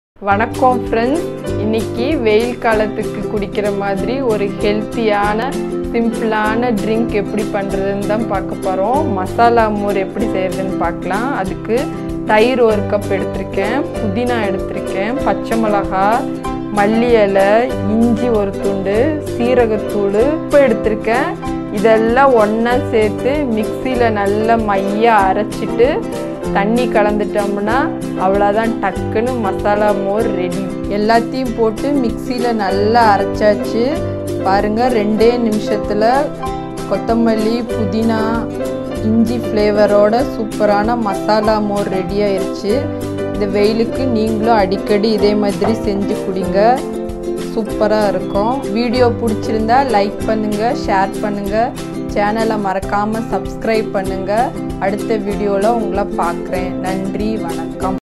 Mor recipe in tamil | Masala mor | Mrs.vadasatti
An ANCIENT DRINK IN TAMILNADU.
HAPPY COOKING.
KEEP THE HAND WASHING AND CLEAN AND FOLLOW THE INTRUCTIONS BY GOVERNMENT TO AVOID CORONO VIRUS.